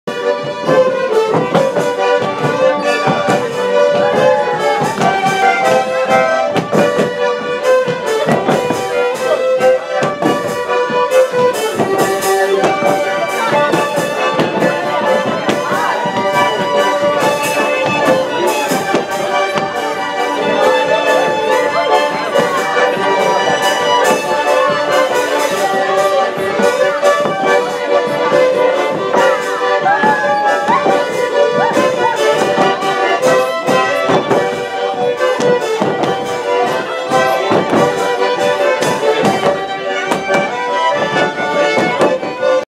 Беларуская школа этнічнай культуры Гуда у Мінскім мястэчку
У Школе традыцыйнага мастацтва адбылася вечарына, прысвечаная заканчэнню першага курсу "Традыцыйны беларускі танец"